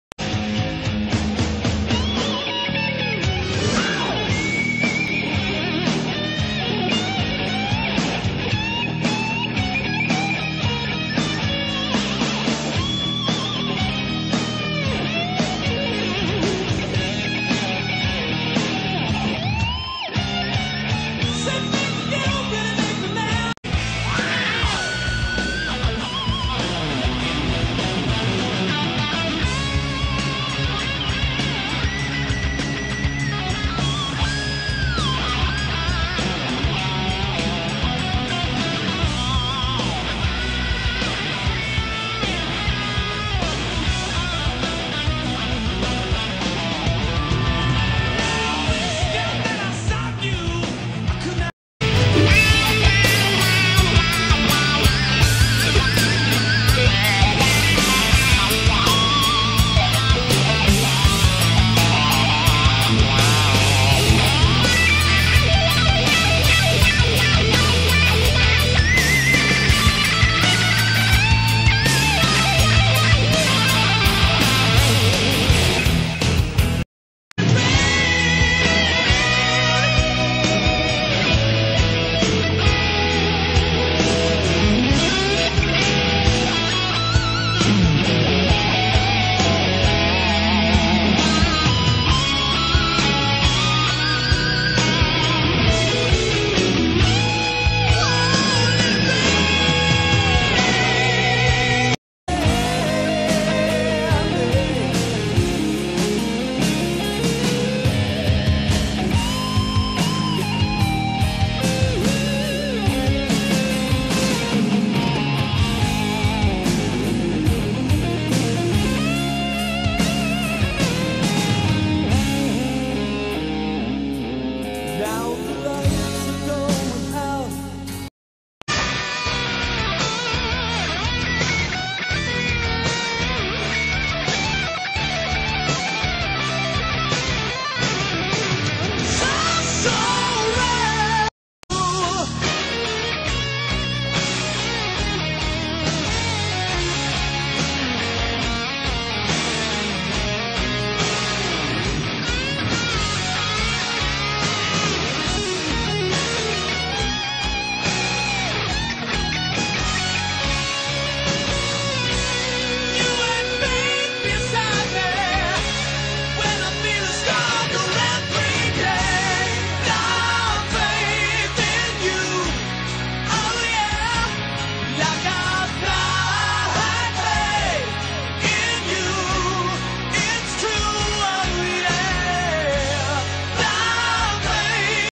A Tribute to WARRANT/JANI LANE And JOEY ALLEN & ERIK TURNER Guitar solos with photo slide for fans.
7Favorite ClassicGuitaristpic. Jani lane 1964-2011) hriatreng nan Warrant guitarist pahnih guitar solo thenkhat hi an thlalak nen thlir mai2 ang aw niang.